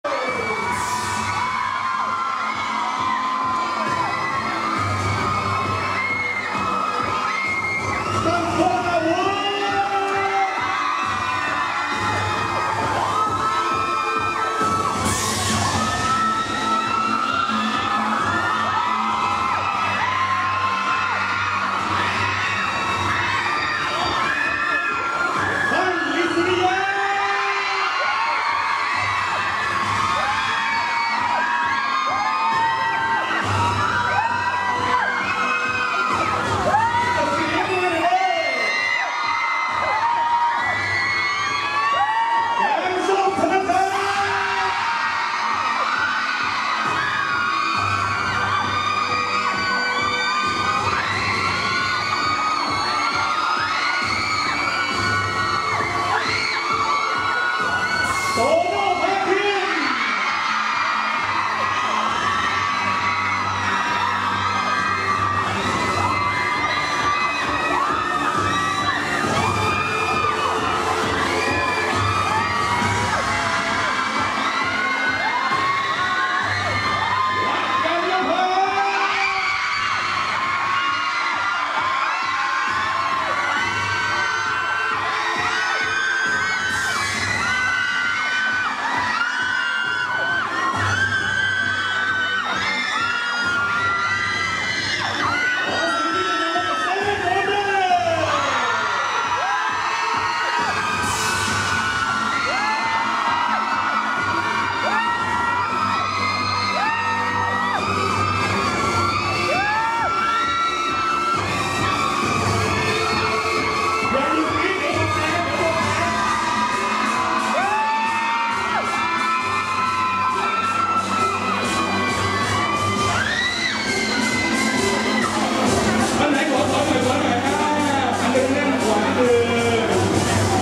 เปิดตัว 7 หนุ่ม
งานแถลงข่าวเปิดตัว 7 หนุ่ม 7 wonders concert

ฮั่น อิสริยะ
แกงส้ม ธนทัต
กั้ง วรกร
กัน ณภัทร
ริท เรืองฤทธิ์
โตโน่ ภาคิน
ตั้ม วราวุธ

ชั้น 4 รัชดาลัย เอสพลานาด วันที่ 25 พค 2558